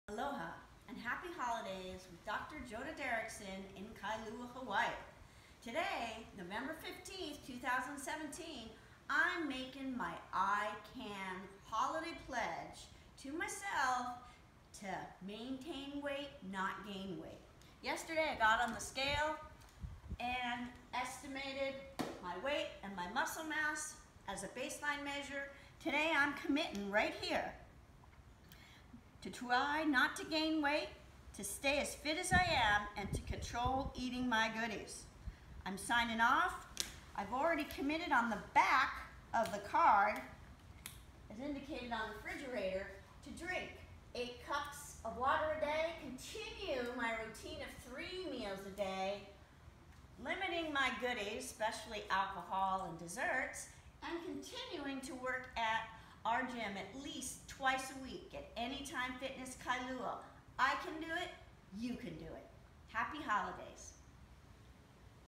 Ican! Holiday Pledge to maintain, not gain weight
Dr. Joda P. Derrickson share her Holiday weight neutral goal and how she plan to accomplish it using ican!